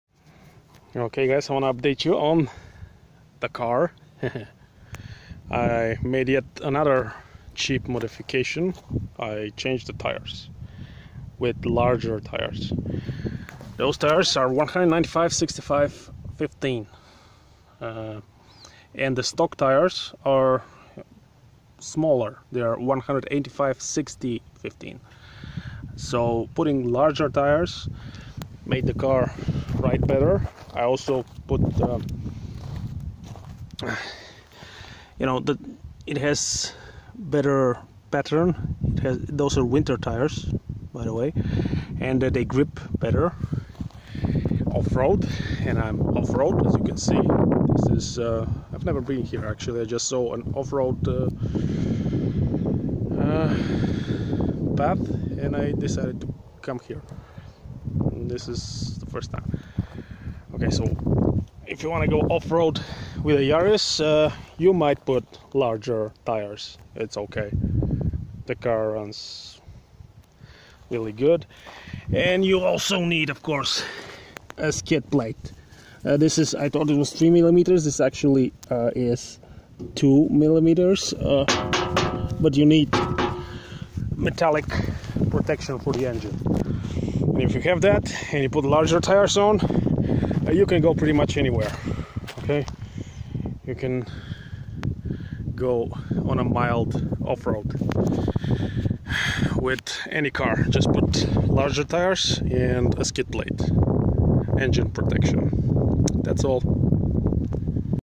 Upgarde Toyota Yaris: Mud and Snow Tires off road for cheap.
Bought a set of 4 used tires for 35 euros. 8.75 EUR a piece :-)